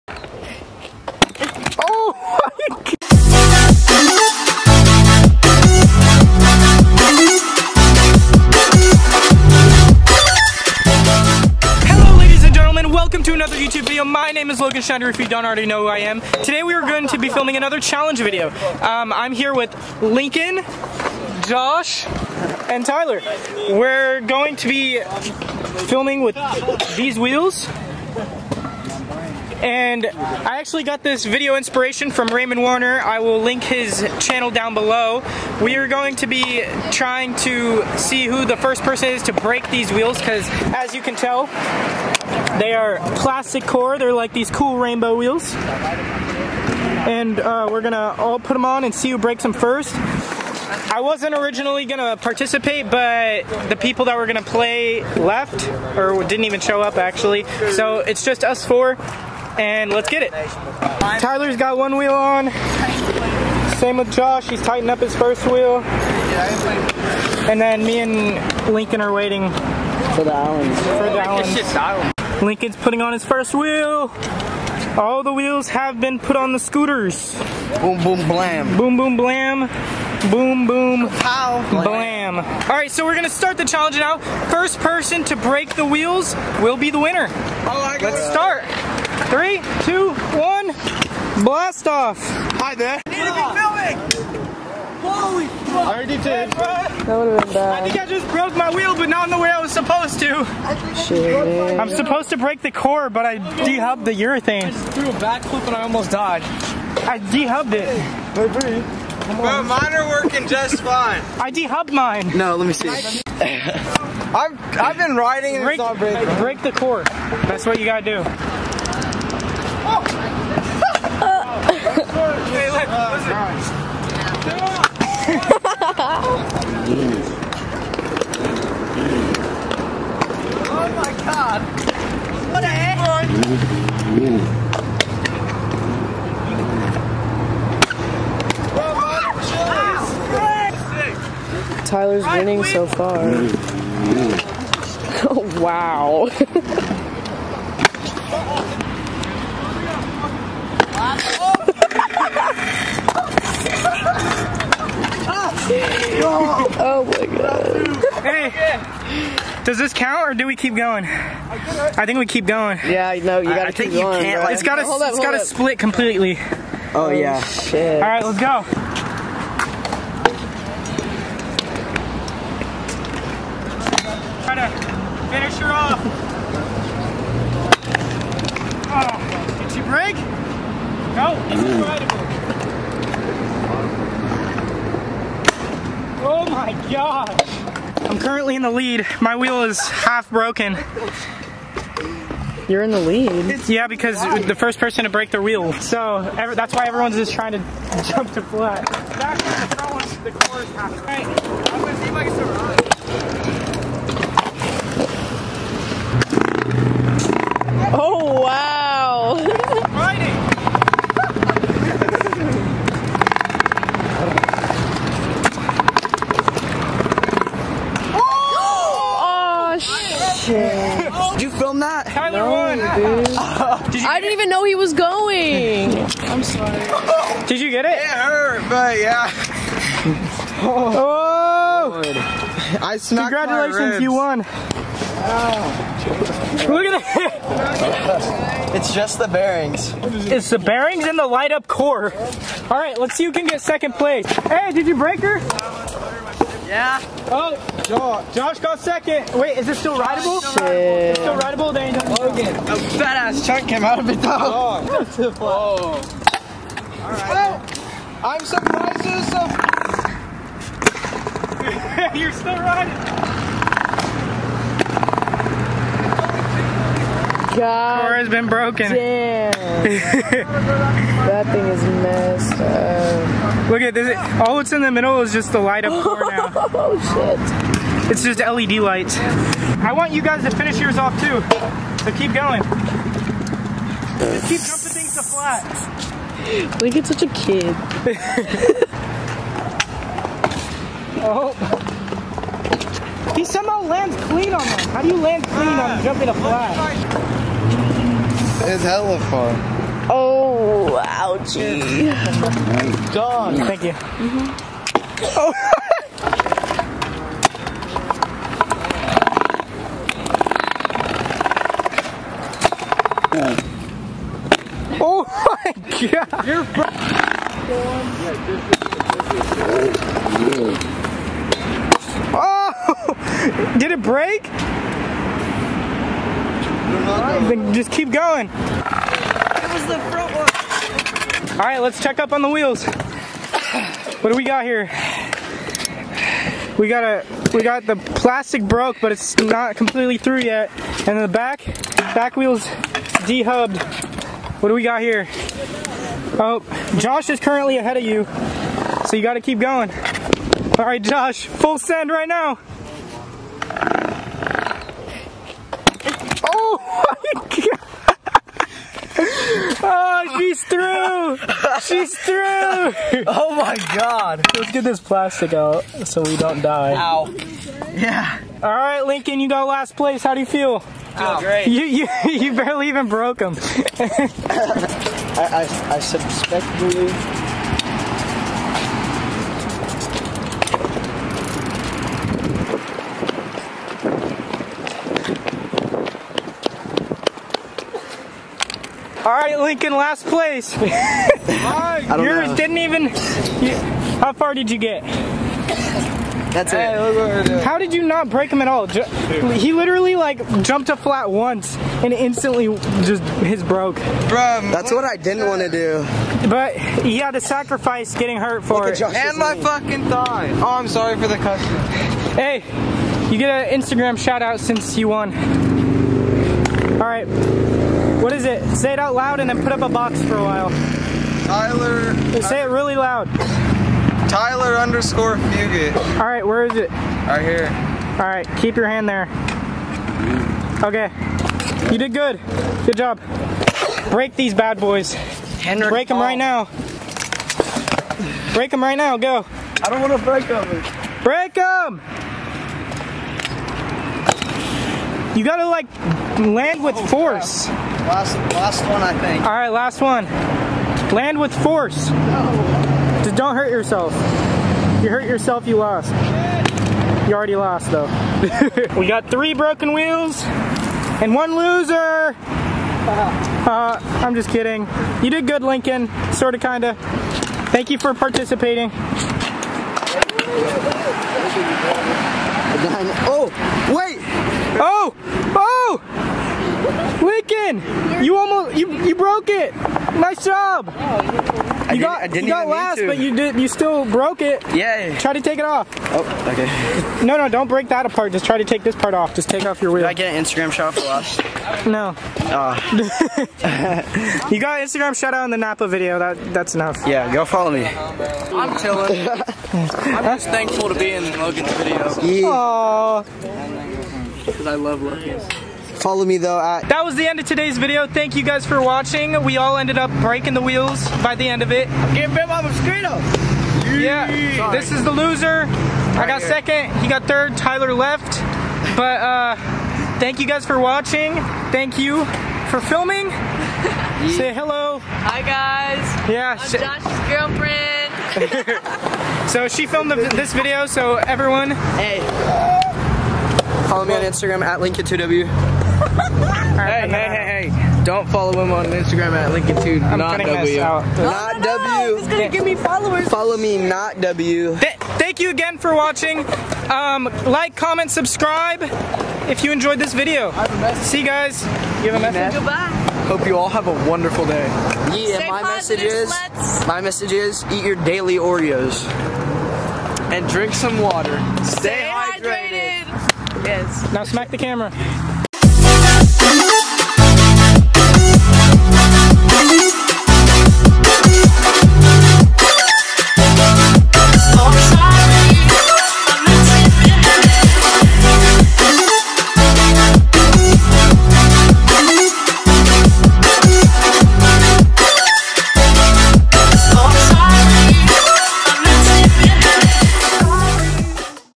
RAINBOW WHEEL DESTROY CHALLENGE!
I had a bunch of these rainbow colored wheels in my garage and I thought I would make a video out of them. I have seen Raymond Warner do a similar video so I will link his channel below.